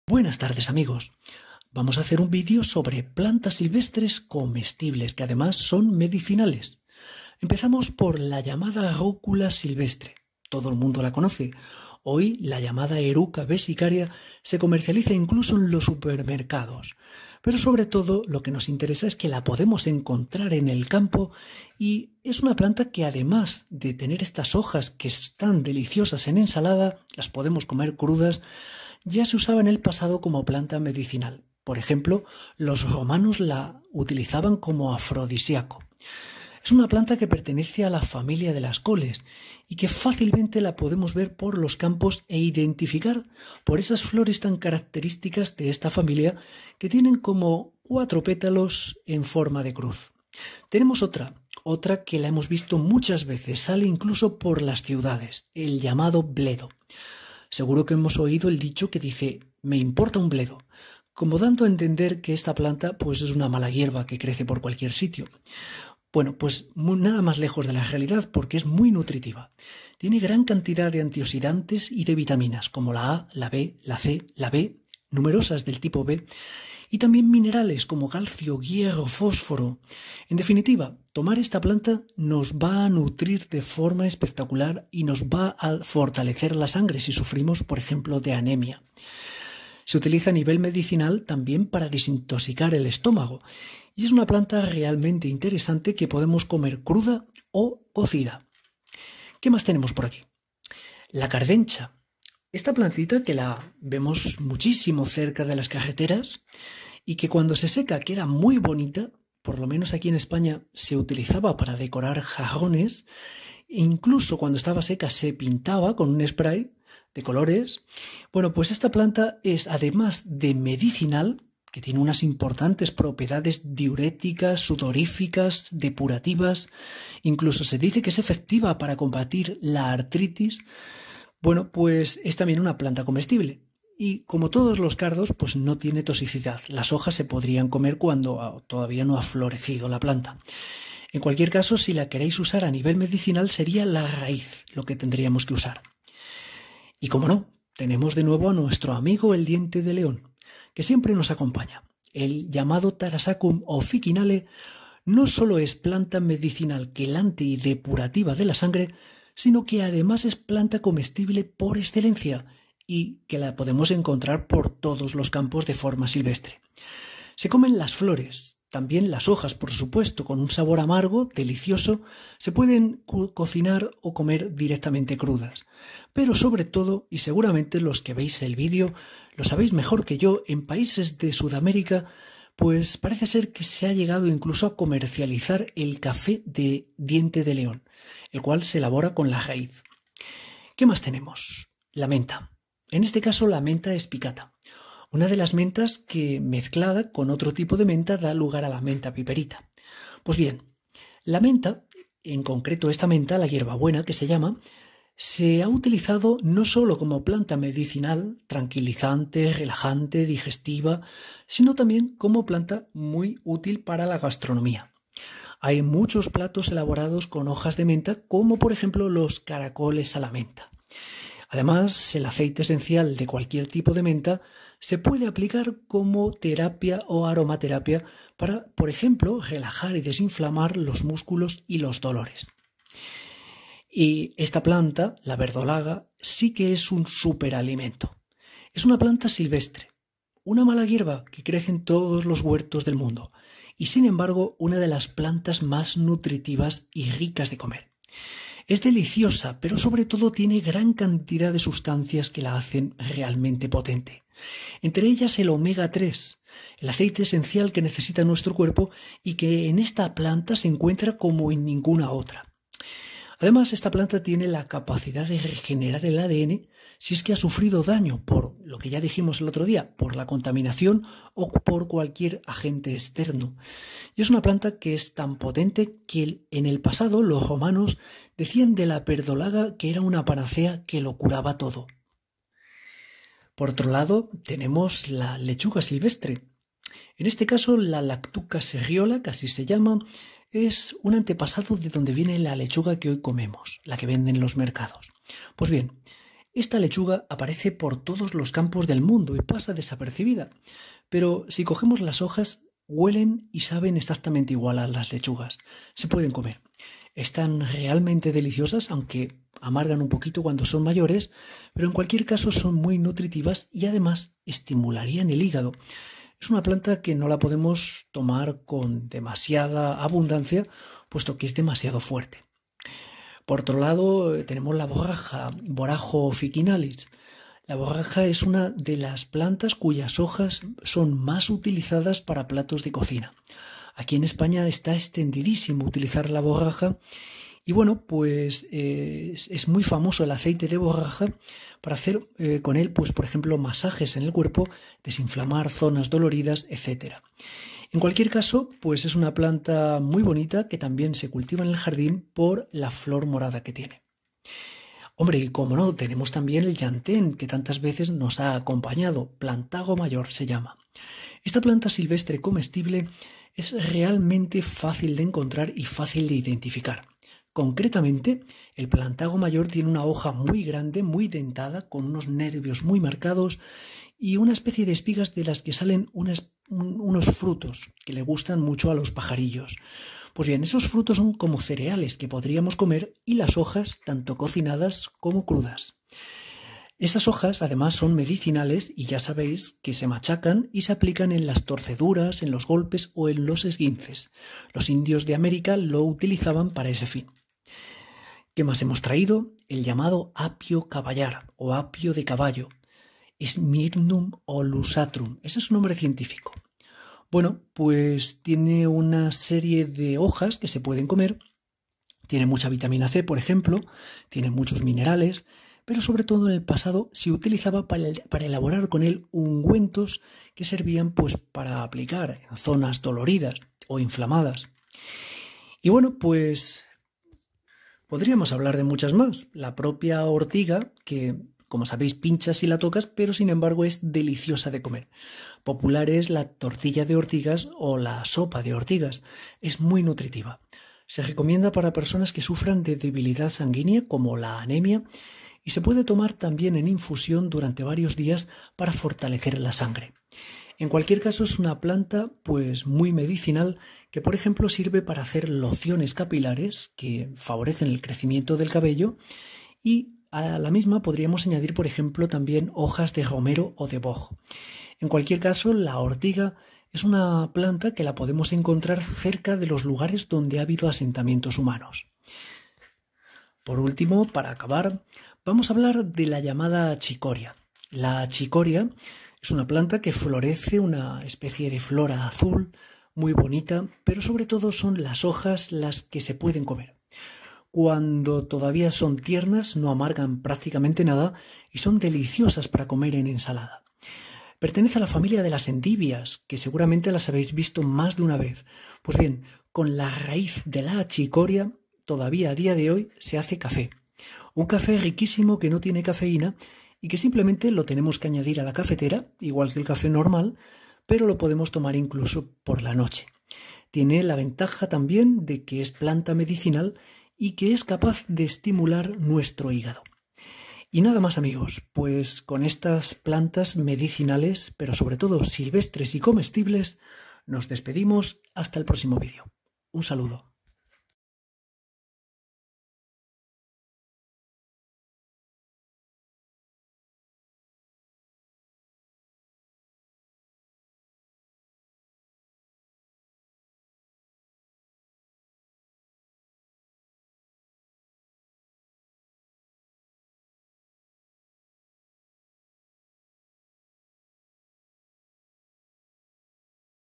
PLANTAS MEDICINALES COMESTIBLES Verdolaga, Llantén, Plantago, Borraja, Cardencha, Rúcula, Achicoria
Taraxacum officinale, de nombre común diente de león o achicoria amarga, pelosilla, corona de fraile, achicoria amarilla, achicoria silvestre, bufas de lobo, chinita de campo, flor de macho, frango, lechiriega, taraxaco y tatusia.

El apio caballar (Smyrnium olusatrum)

Amaranthus , bledo,

rucula, Eruca vesicaria, llamada comúnmente arúgula, oruga, ruca, rúcula o roqueta

La ortiga (del latín 'urtica') es el nombre común de las plantas del género Urtica,

Cichorium intybus, la escarola o achicoria común,  abalea (2), achicorea, achicoria (48), achicoria amarga (en Bolivia29​ o "philliyuyu"29​ (nombre quechua)29​ o "chancoroma"29​ (aymara)29​)(6), achicoria borde, achicoria brava, achicoria de balea, achicoria de burro, achicoria de hojera, achicoria negra, achicoria silvestre (en Argentina30​29​ fue propuesto como nombre patrón para la especie por Petetín en 1984.31​ O